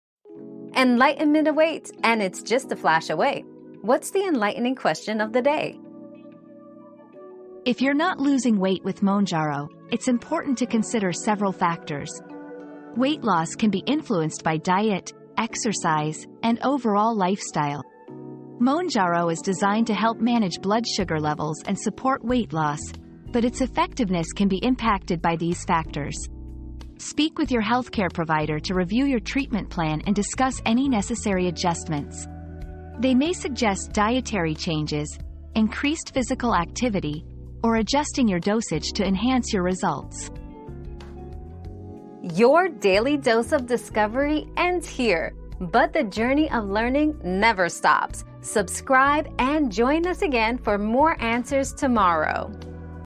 Why is my Mounjaro not losing weight?
Unlocking Weight Loss with Mounjaro: What You Need to Know 👉 Unlocking Weight Loss with Mounjaro 👉 Discover why your weight loss journey with Mounjaro may be hitting a roadblock and how to overcome it. Learn about the factors that influence weight loss, including diet, exercise, and lifestyle choices. Find out how Mounjaro can help manage blood sugar levels and support weight loss, and why it's important to consult your healthcare provider for personalized adjustments to optimize your results.

Laura S. Harris (2024, February 7.) Why is my Mounjaro not losing weight?
    🌐 AskAbout.video